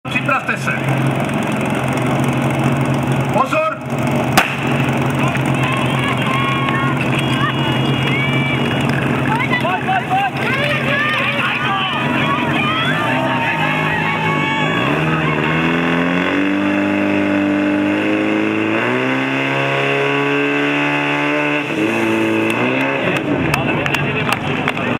SDH Metylovice: Petřvaldík ženy v Mošnově 2012 - 1. kolo
Mošnov, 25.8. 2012, 13. kolo Moravskoslezské ligy v požárním útoku